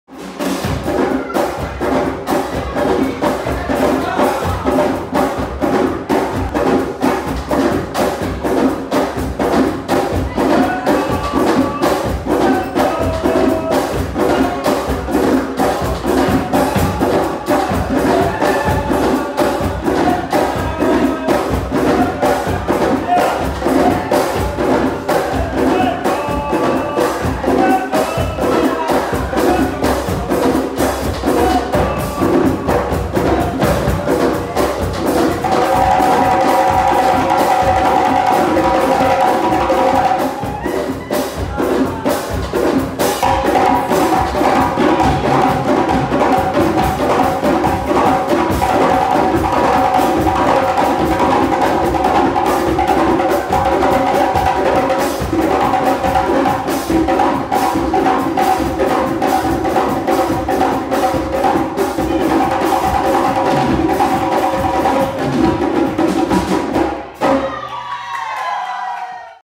African Drumming 2
Drums and Djembe Jam in Gambia :
Ueli Müller  Drums
Ibrahimi  Djembe